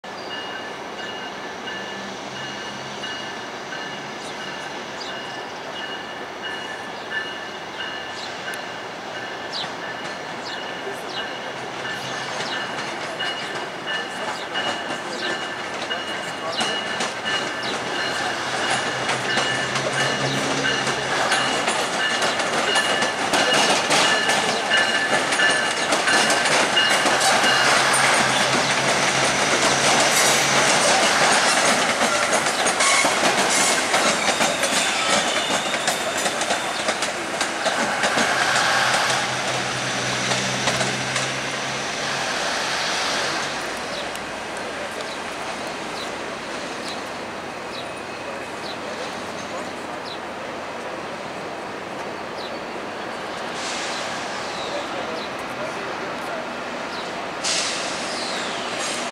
Union Pearson Express Train Departs Union Station
A three car Union-Pearson Express (UPE) train, consisting of #1003, #3003, and #1006, is seen departing Toronto's Union Station enroute for Lester B. Pearson Airport on June 15th, 2022.